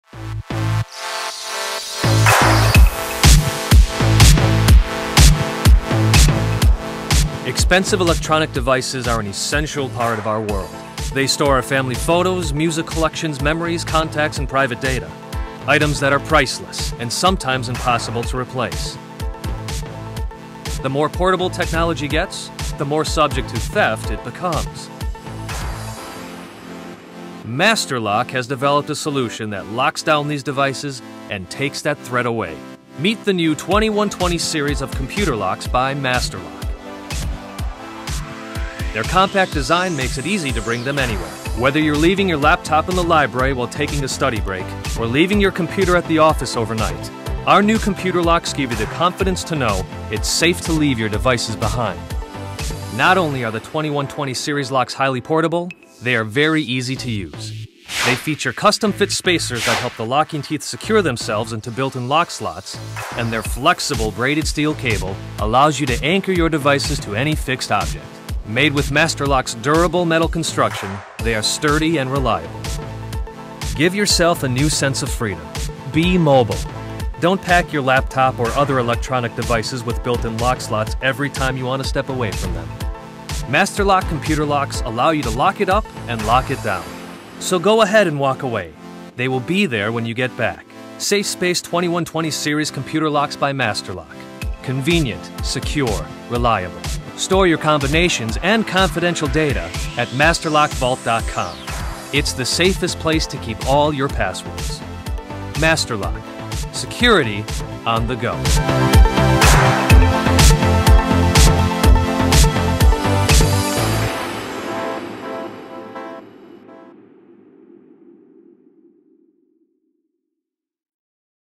2120D Computer Locks
The 2120D & 2120DWD SafeSpace® Computer Locks secure any electronic device with a built-in lock slot.